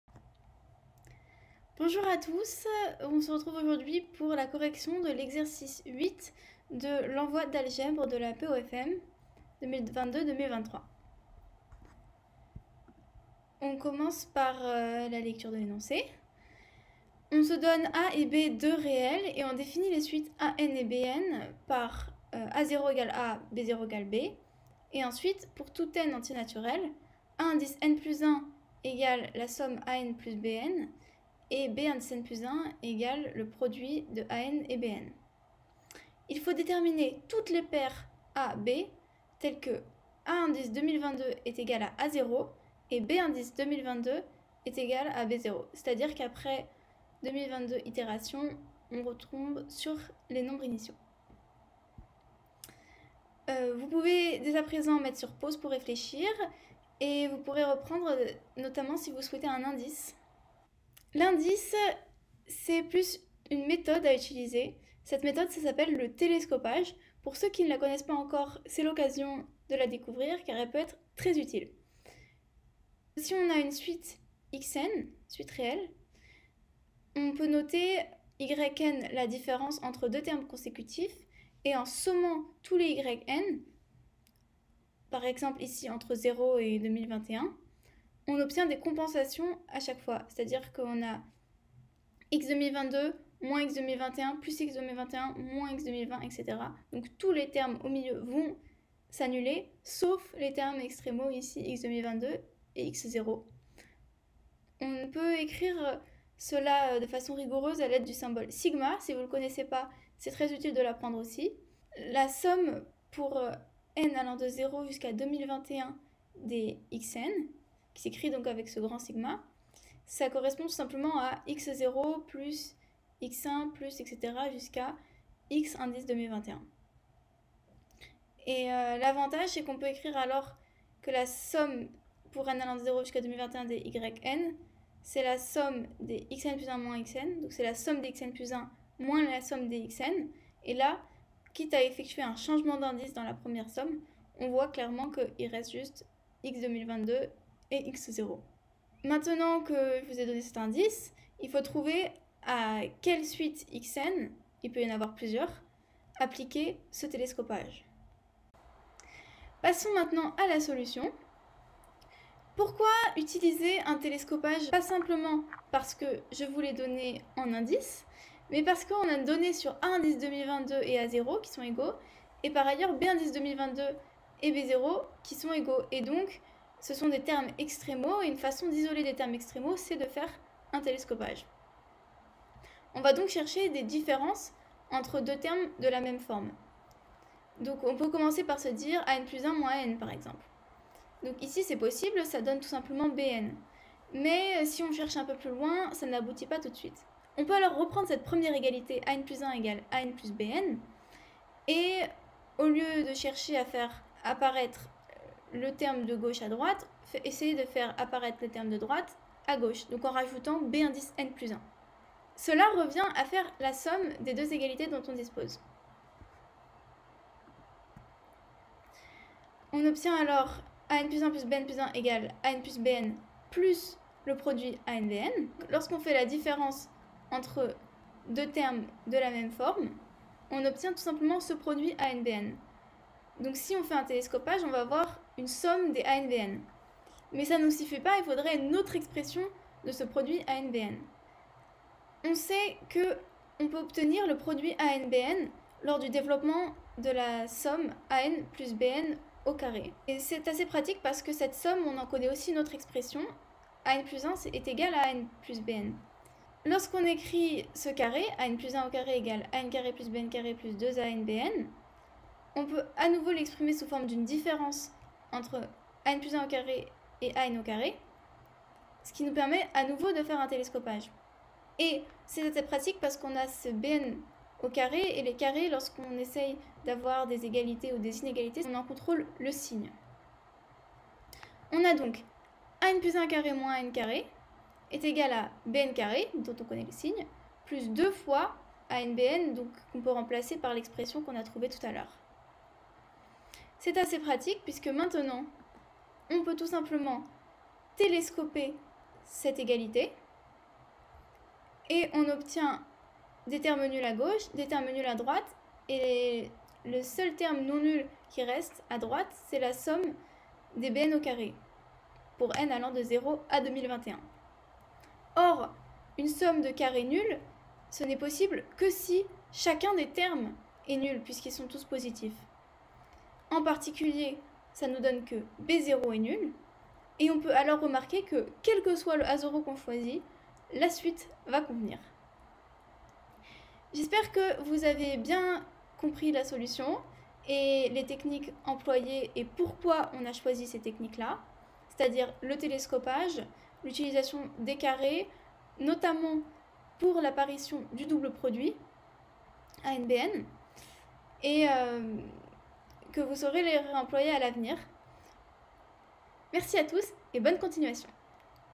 Envoi d'Algebre, 2022-2023 Exo 8 [Alg-C-23]
Correction de l'exercice 8 de l'envoi d'algèbre de la POFM 2022/2023